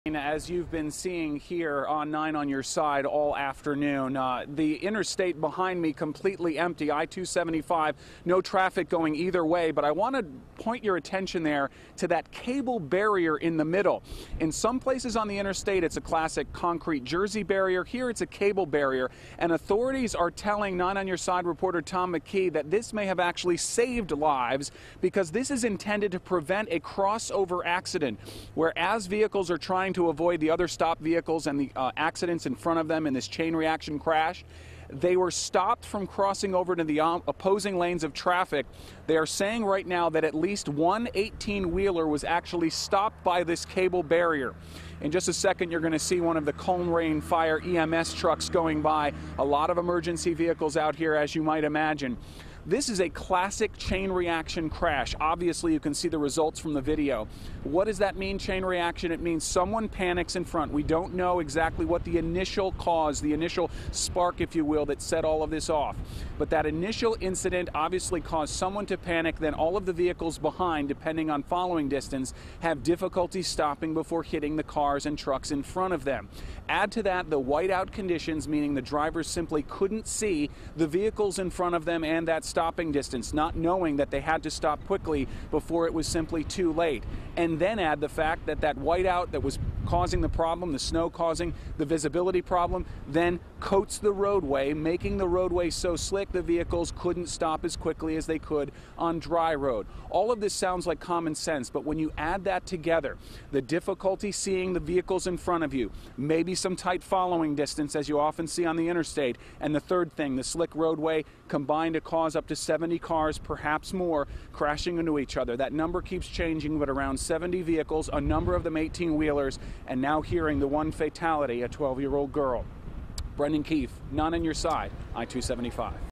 Description of chain reaction crash on I-275
9 On Your Side's Brendan Keefe reports from the road closure near I-275 in Colerain.